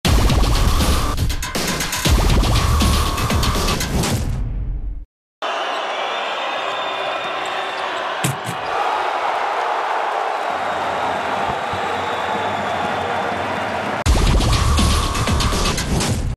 WOW! What a dunk by Marko Simonović! (Mornar - Crvena zvezda mts, 31.10.2016)
Marko Simonović lifted the crowd on their feet with a brutal dunk in the game against Mornar in Bar.